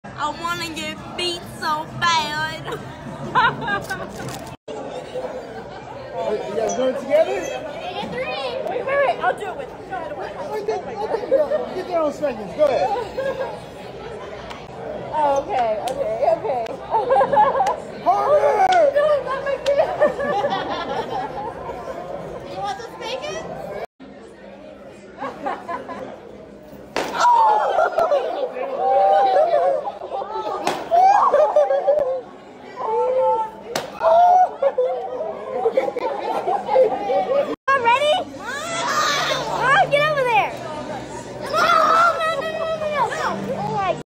Just Nice Family in Vegas at Heart Attack Grill.... getting spanked!
This is a clip from our family road trip.  We traveled to 6 different states, and this is one of our stops in Vegas.  We went to the Heart Attack Grill and got spanked, well almost all of us!  LOL